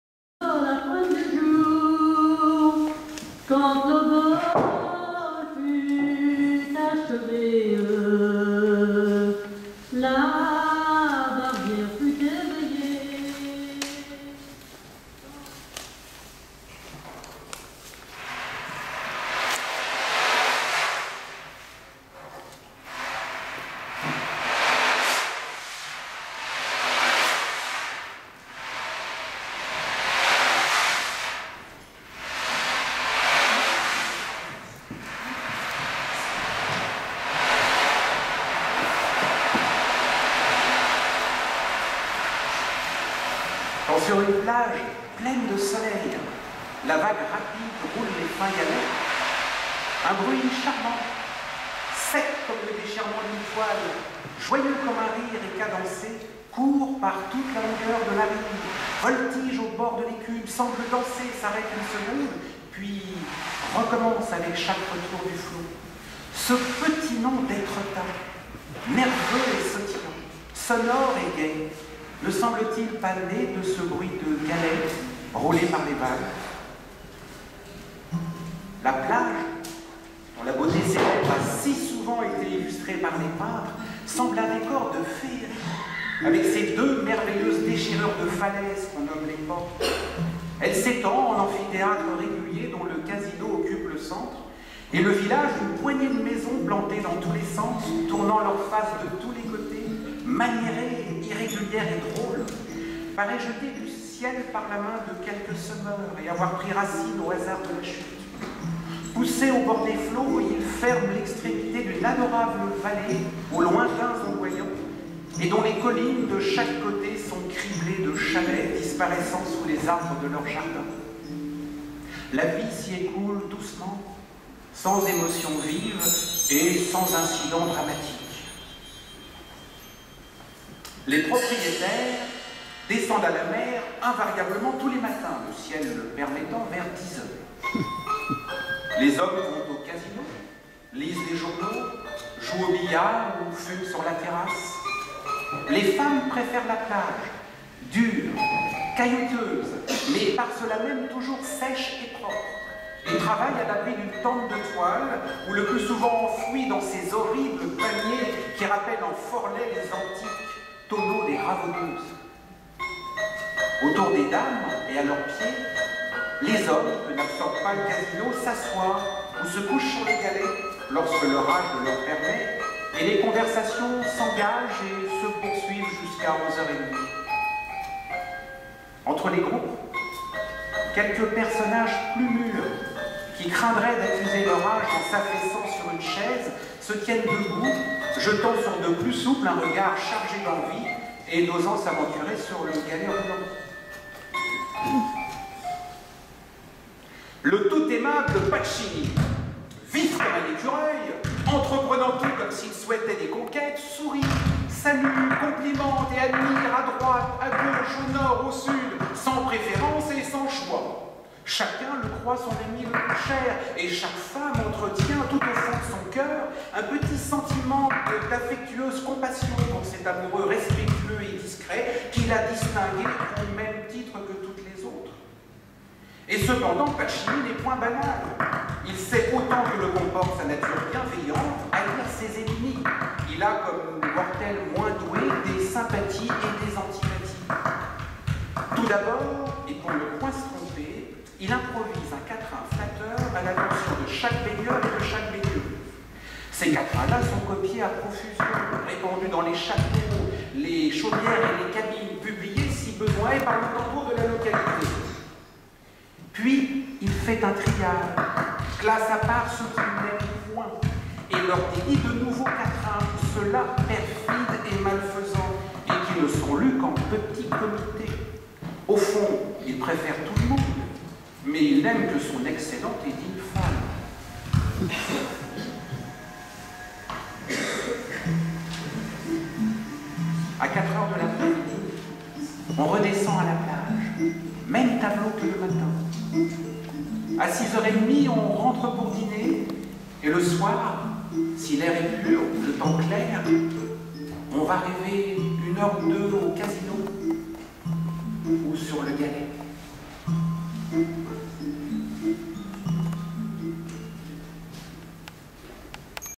Guy de Maupassant
Récitant Philippe Bertin
Musique Marion Motte
Etretat (1883)
La Normandie, région natale de Maupassant, tient une place importante dans son oeuvre. Dans cette nouvelle, il plante le décor d’une ville qu’il adorait : Etretat. A neuf ans, ce fut son terrain de jeu et d’apprentissage, entre mer et poésie.
Son oncle Alfred, poète, était le meilleur ami de Flaubert. A 33 ans, ses droits d’auteur lui permirent d’y faire construire une maison « La Guillette » où il achèvera l’écriture de « Bel ami ».